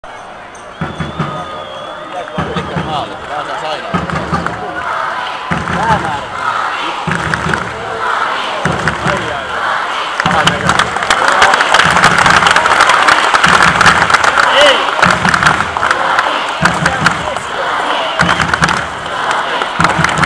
UEFA Women's Euro 2009 , Finland - England crowd in Veritas Stadion
Short clip of crowd in Kupittaa Veritas stadion